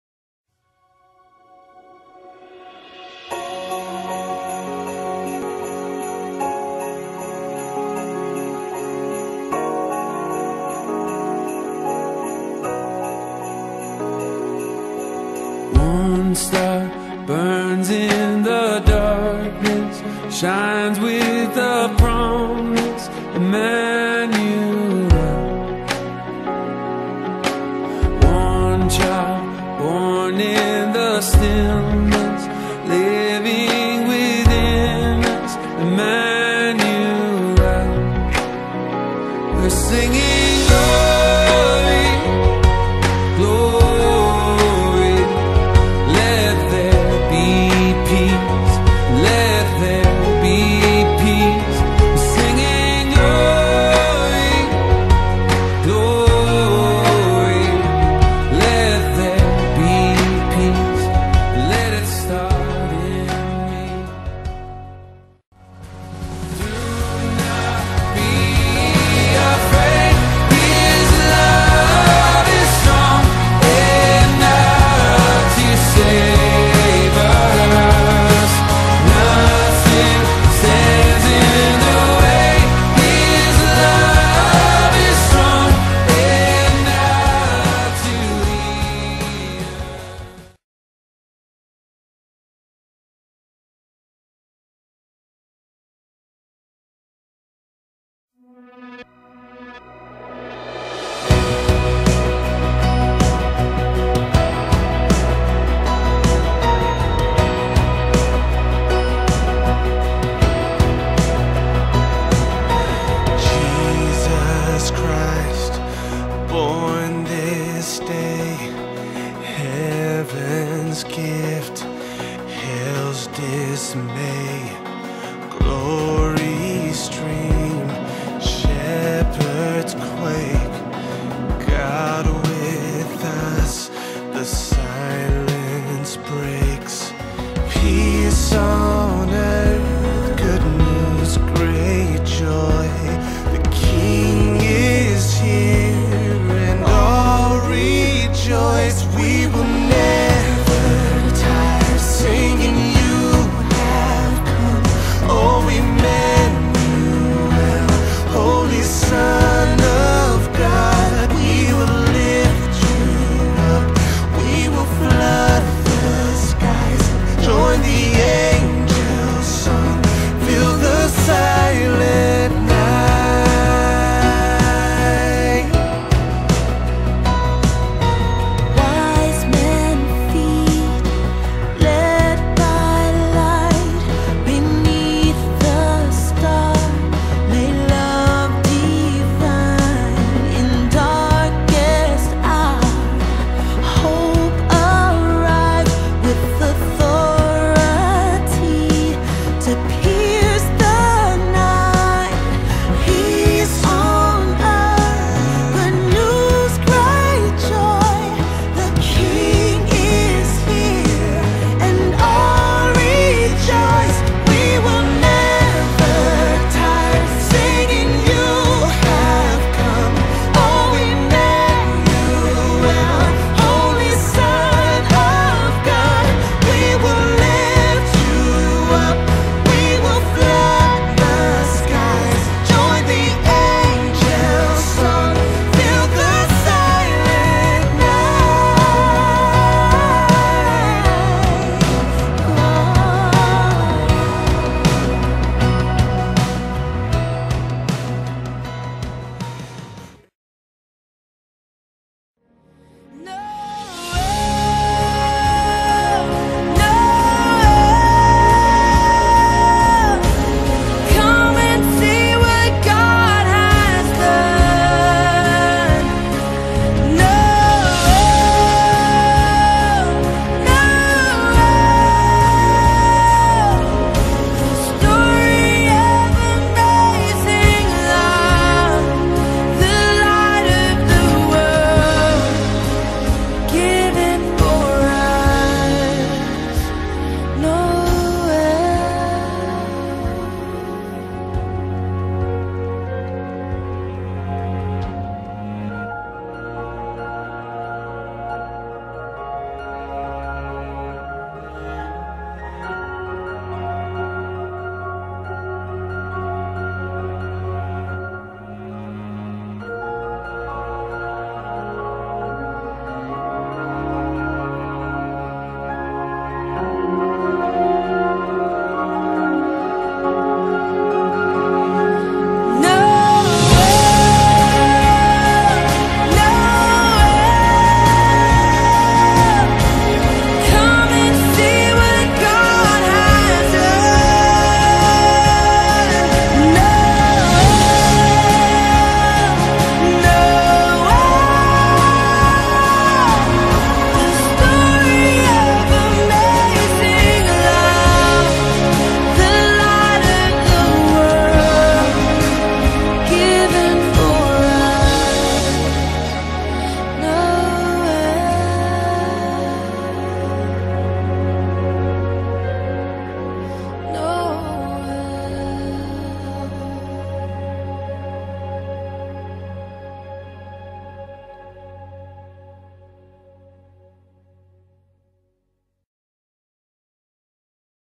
New Covenant Fellowship Church Living Nativity
December 16-18, 2016 - A recap of nights 1 and 2 of our Living Nativity at New Covenant Fellowship Church.  God moved in mighty ways and the Message was shared with many, not by any of our own accord, but by His mighty works.